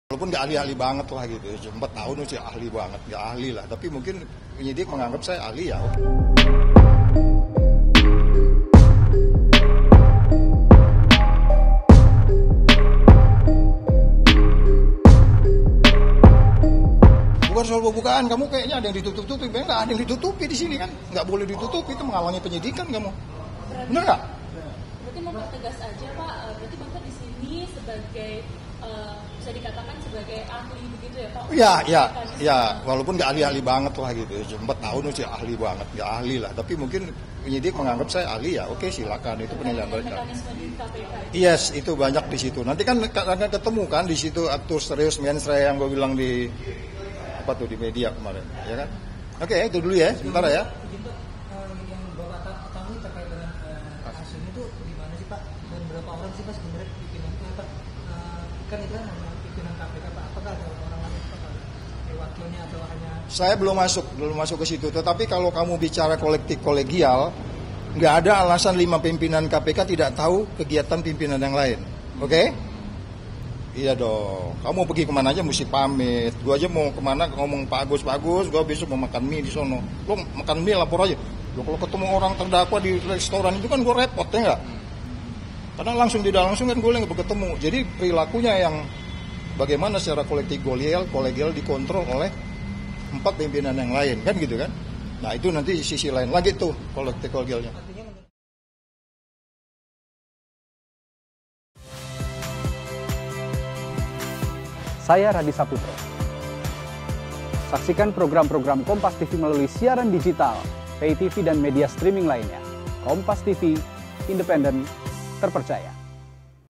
Eks Pimpinan KPK Diperiksa soal Kasus Pemerasan SYL
JAKARTA, KOMPAS.TV - Wakil Ketua KPK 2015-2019 Saut Situmorang diperiksa di Polda Metro Jaya, Selasa (17/10/2023).

Saut mengaku bahwa dirinya memenuhi panggilan penyidik sebagai saksi ahli dalam kasus dugaan pemerasan terhadap SYL oleh pimpinan KPK.

"Iya walaupun enggak ahli-ahli banget lah. Tapi mungkin penyidik nganggep ahli ya oke silakan," kata Saut kepada wartawan, Selasa (17/10/2023).